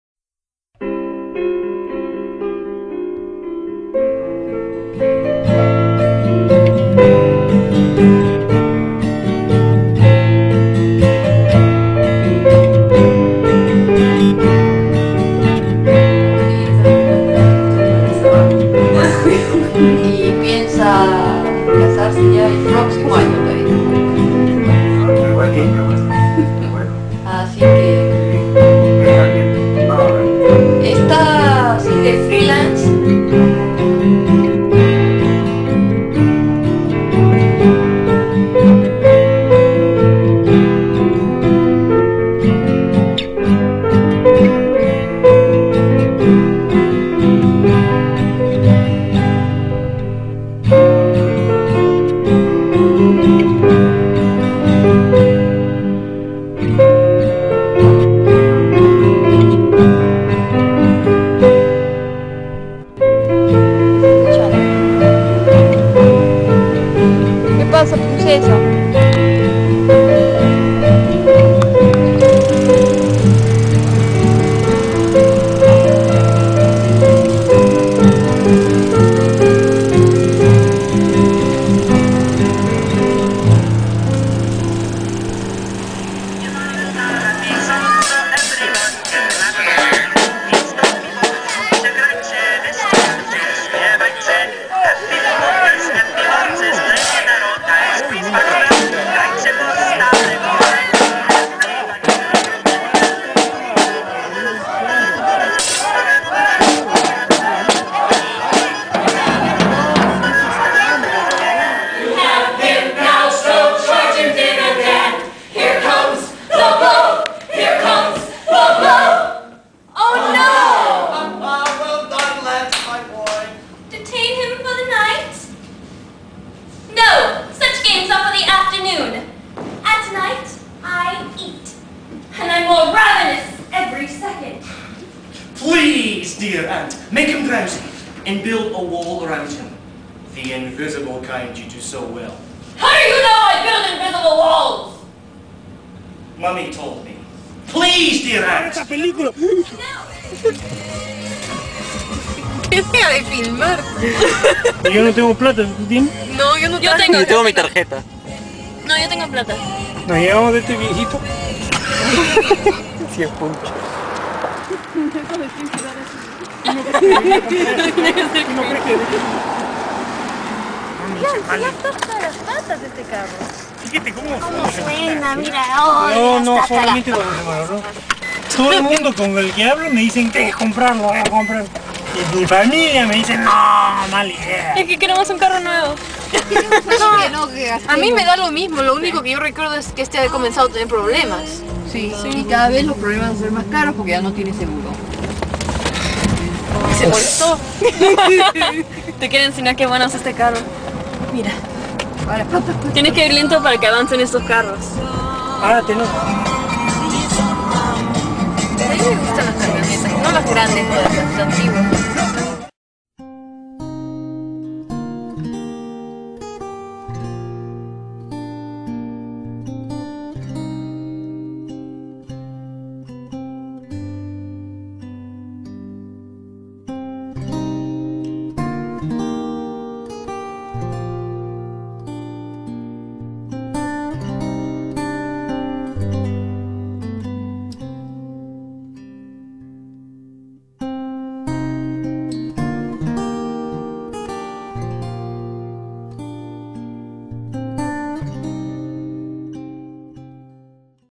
Michigan Summer 2007
Video of Michigan during June-Juy 2007. Starts with theme composed by myself and then has some shots taken during the trip.